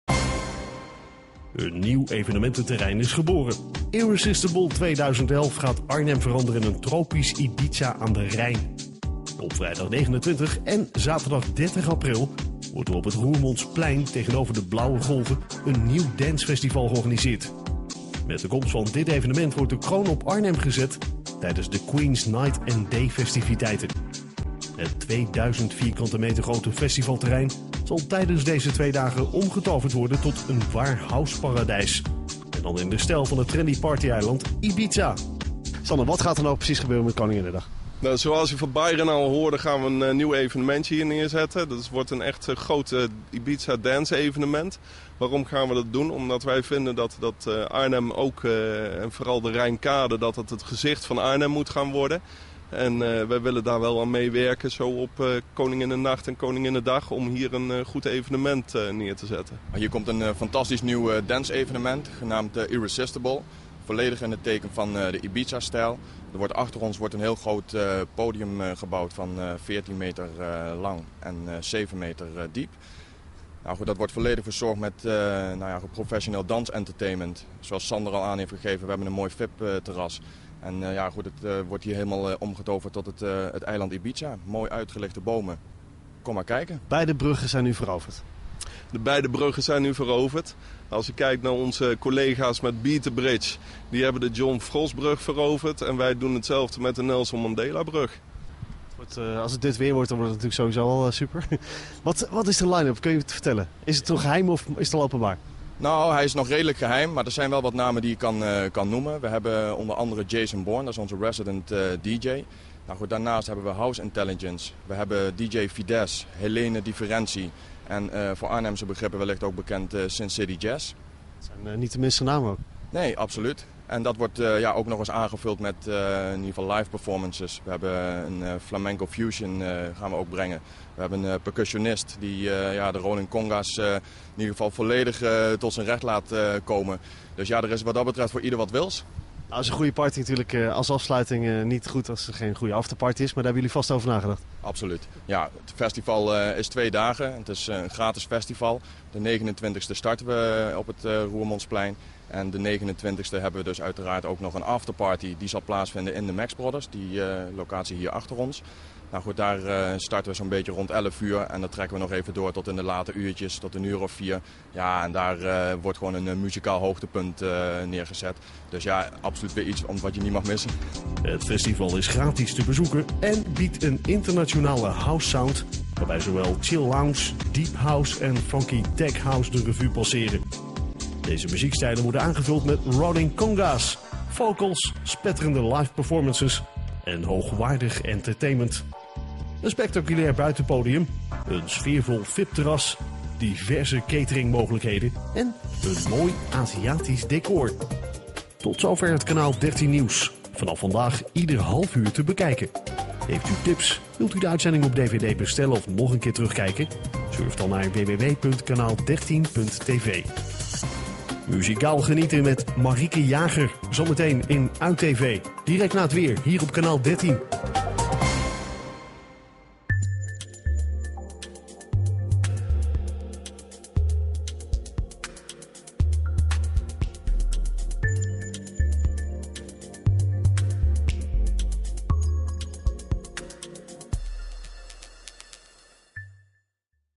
IBZ Code festival IRRESISTIBLE 2011 Kanaal 13 intervieuw
Kanaal 13 intervieuw van 11 april 2011 inzake IBZ Code festival IRRESISTIBLE 2011